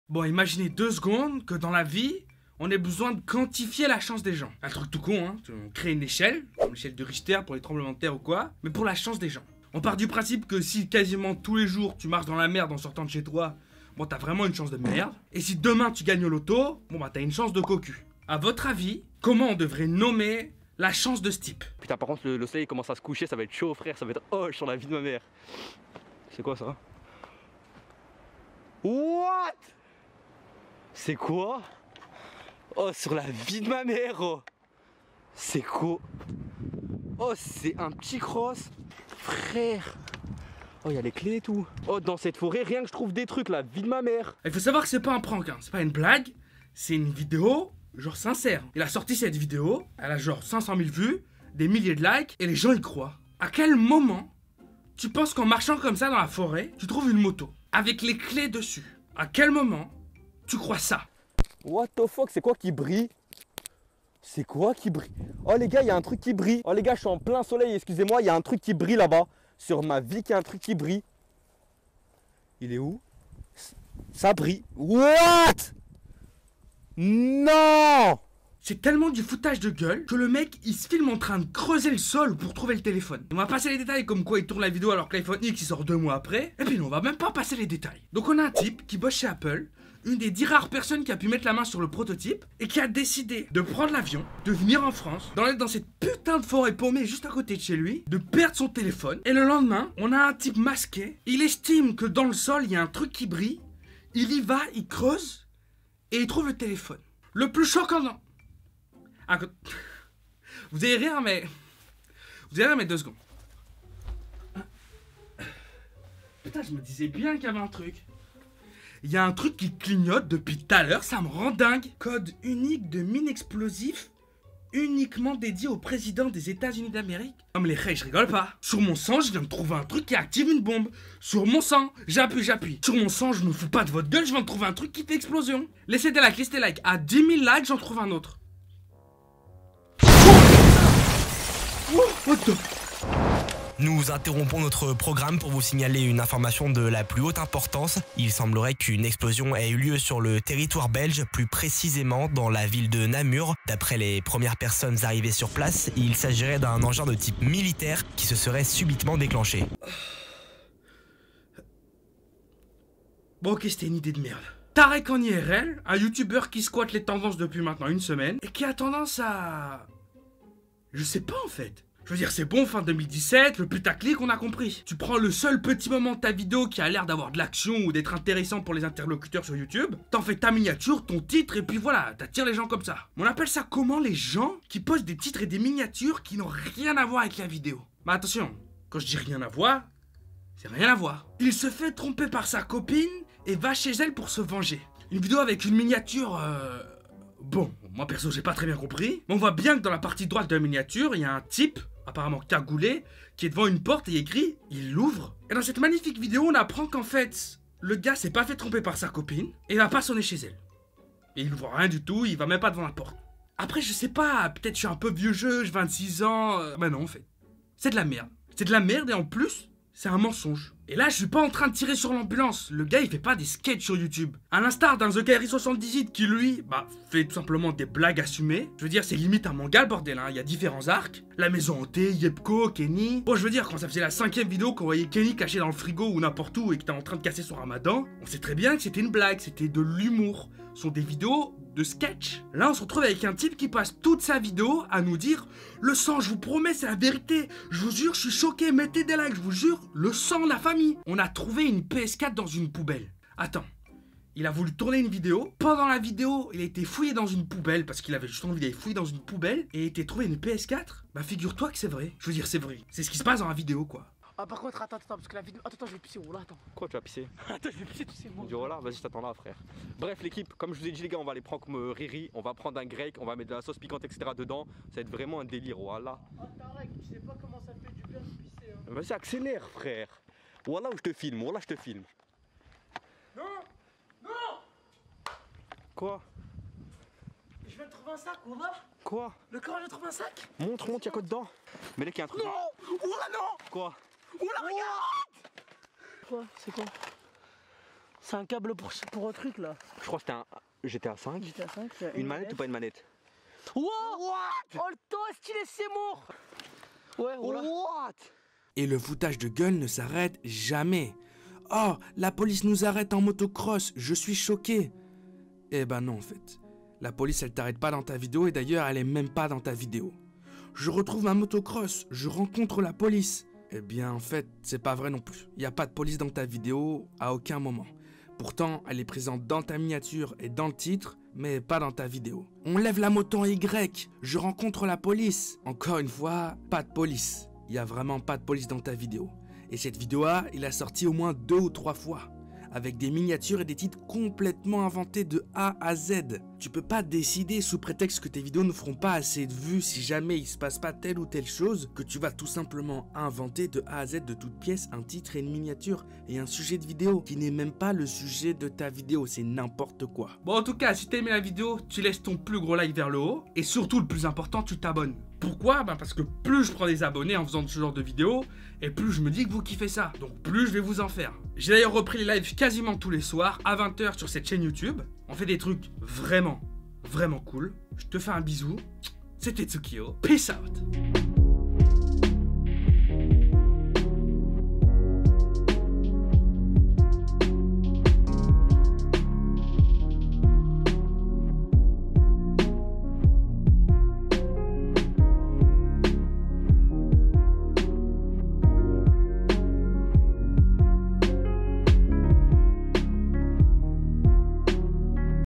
Tarek en IRL - Quand le fake dépasse l'entendement
Merci à Flo pour avoir prêté sa voix : florian.radio sur snap